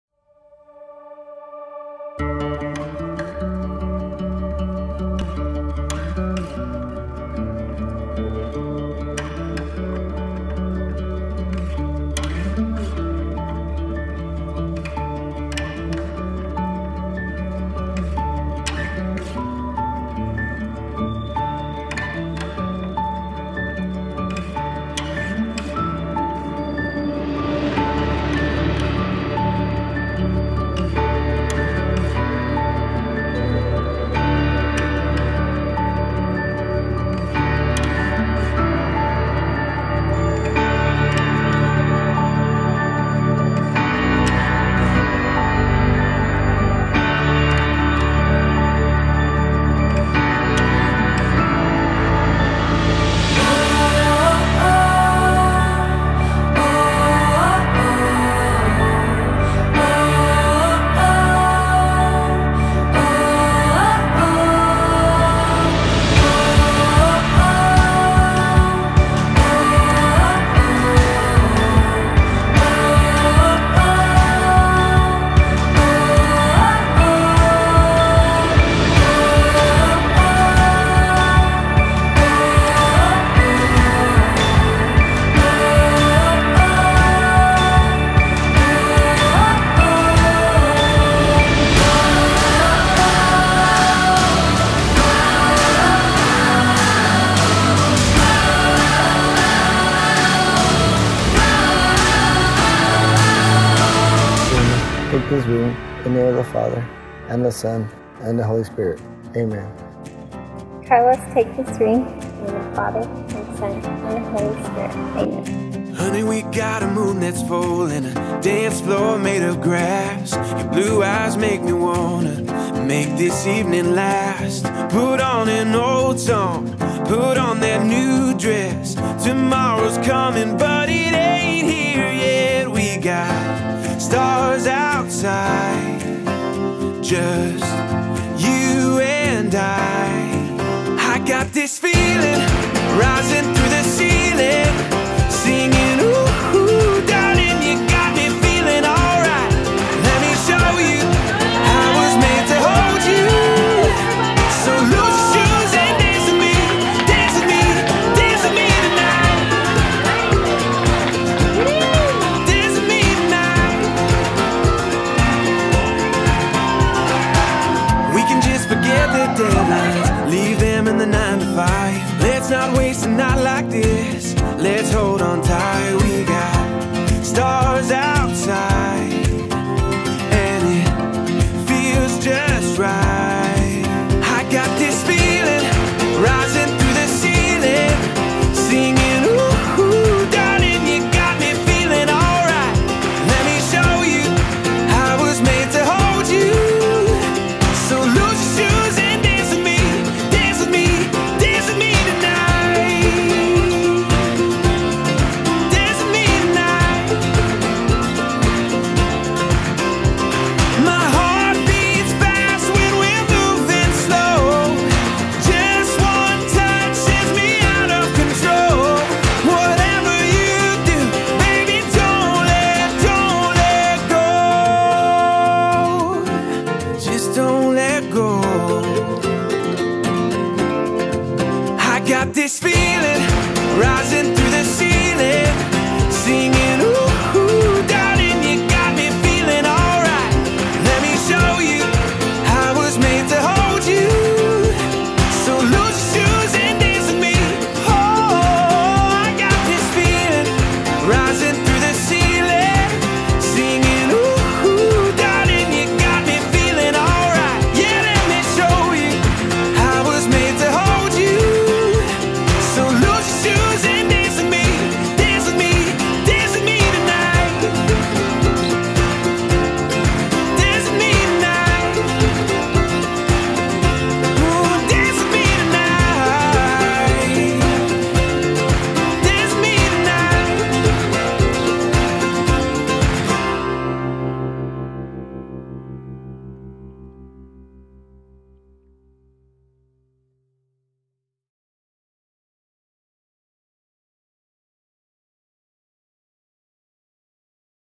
Carlos & Irma Highlights
Carlos and Irma were married in a traditional catholic ceremony, followed by a fun and laid-back reception at Catalina State Park.
 What started as a cool, rainy morning turned into a warm, sunny afternoon.
 FireflyWeddingVideography.com
 Photographer: Kelsey Brown of Lumos Photography